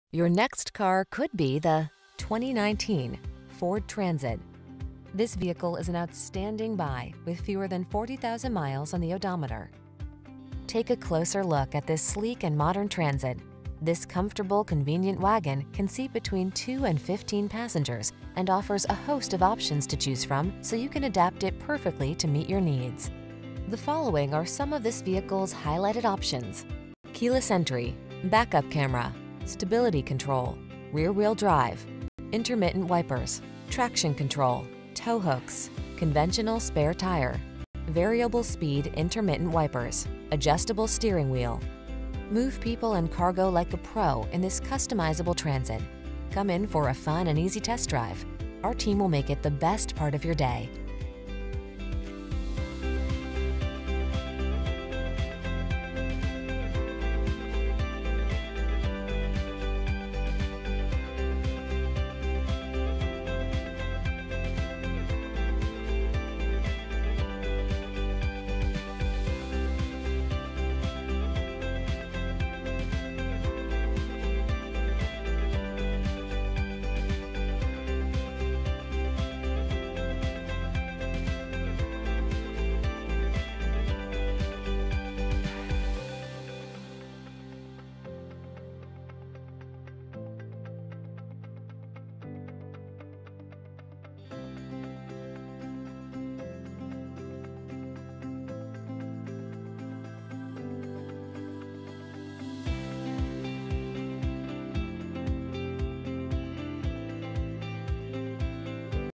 2019 Ford Transit_Van Worthington, Columbus, Westerville, Powell, Delaware PXAB51666
Oxford White Used 2019 Ford Transit_Van available in Worthington, Ohio at Mark Wahlberg Chevrolet of Worthington. Servicing the Westerville, Powell, Delaware area.

700 E. Dublin-Granville Road

Clean CARFAX.Oxford White 2019 Ford Transit-250 RWD 6-Speed Automatic with Overdrive 3.7L V6 Ti-VCT 24V